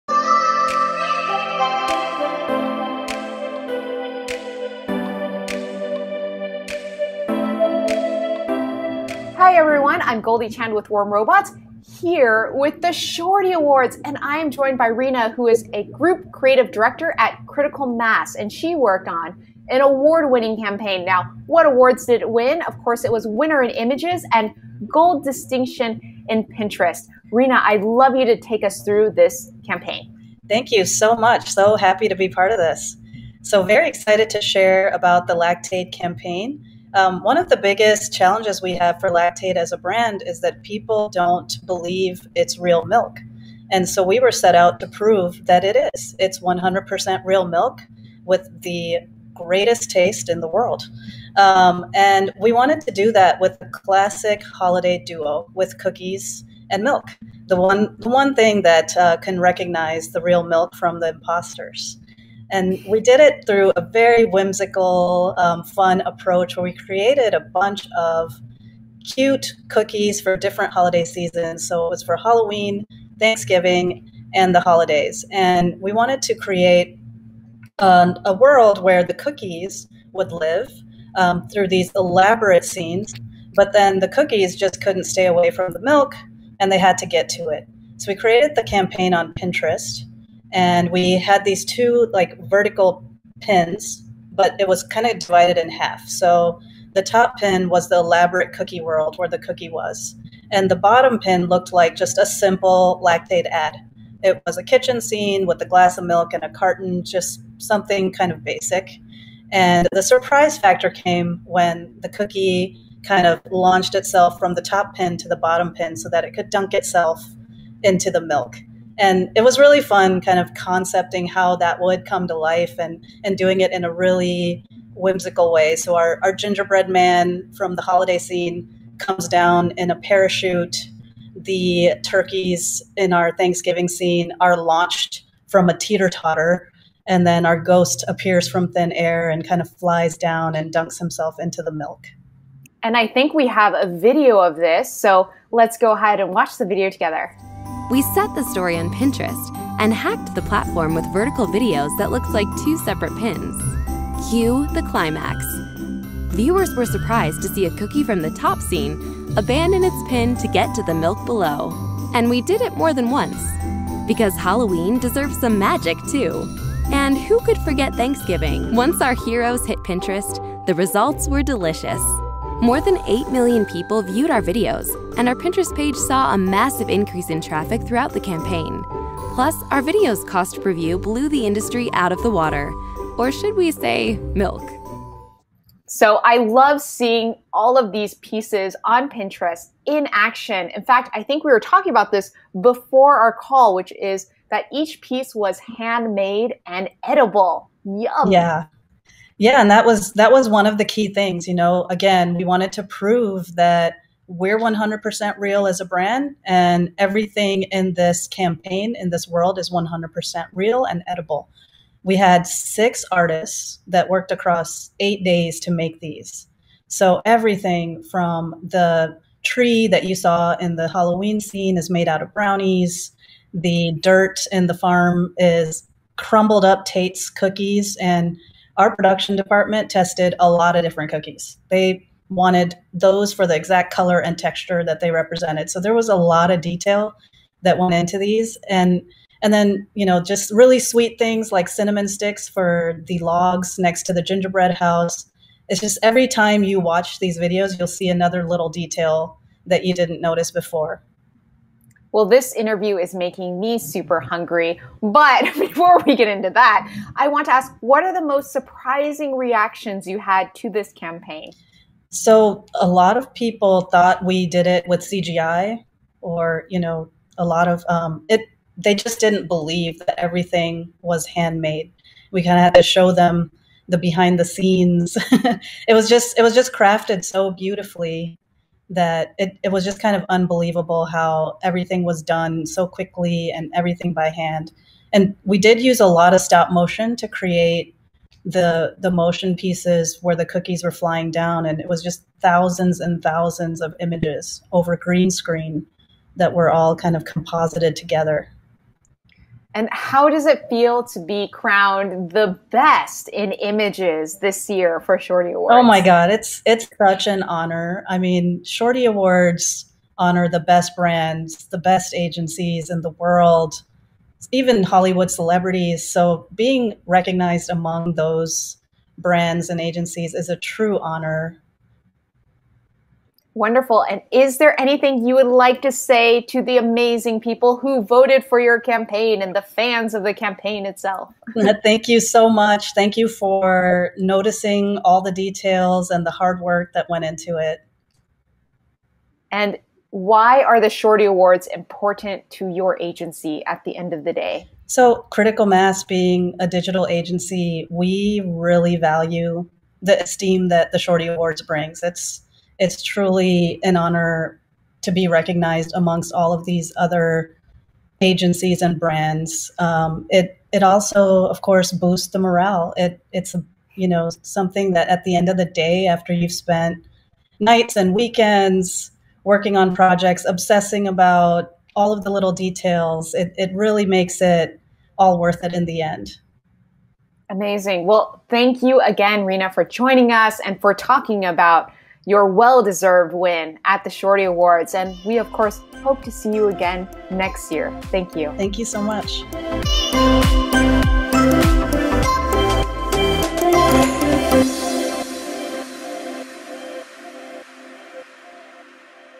Winner's Circle: Best in Images, Critical Mass, Johnson & Johnson
A very warm congrats to Critical Mass, Johnson & Johnson for being honored in the 12th Annual Shorty Awards!